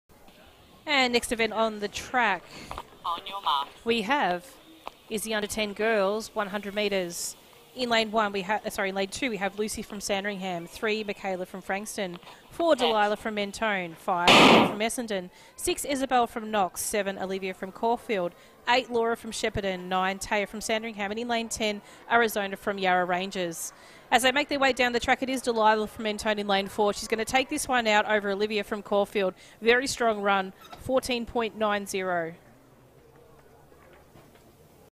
U10 Girls 100m Heat 1: 2022 Commonwealth Bank State Combined Events Championships - Saturday
SHORT DESCRIPTION: U10 Girls 100m Heat 1: 2022 Commonwealth Bank State Combined Events Championships - Saturday